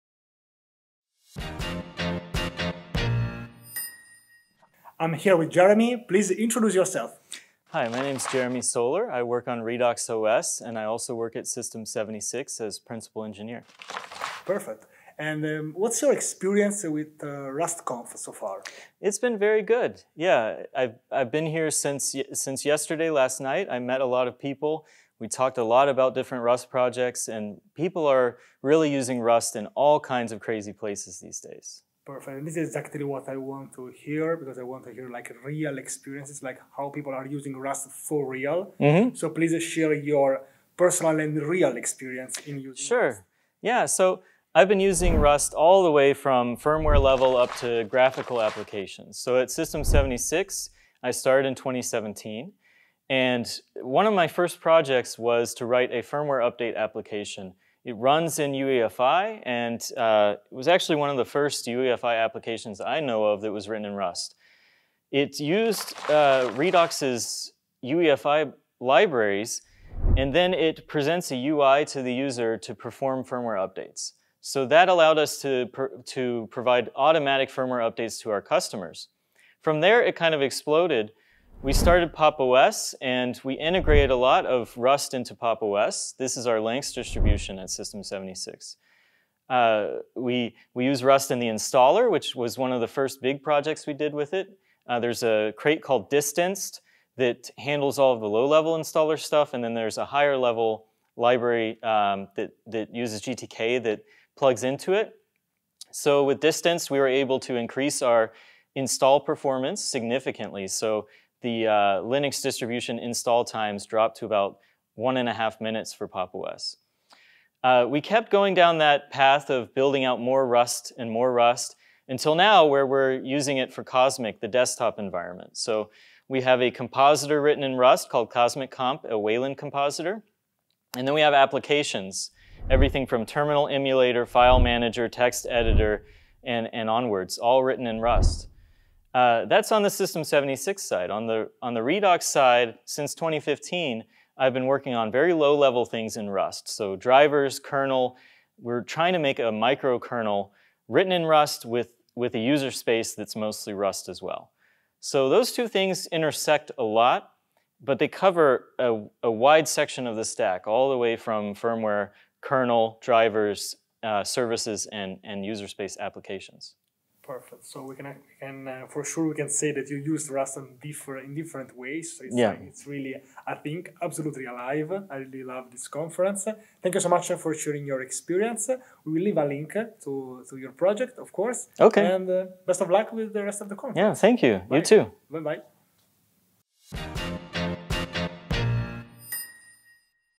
Redox OS: Is the Future of Operating Systems Written in Rust? - With Jeremy Soller
A quick chat with Jeremy Soller, the creator of Redox OS, the innovative operating system written entirely in Rust.

In this 3-minute interview, we discuss the core vision of Redox

📅 Upcoming Events: RustConf 2026: Sept 8–11 in Montreal, Canada. Check it out

🔍 We Discuss:
Why Rust? Soller's reason for choosing Rust for an OS.
The Future: What's next for Redox OS development.

Redox OS Project: [Link to the official Redox OS website]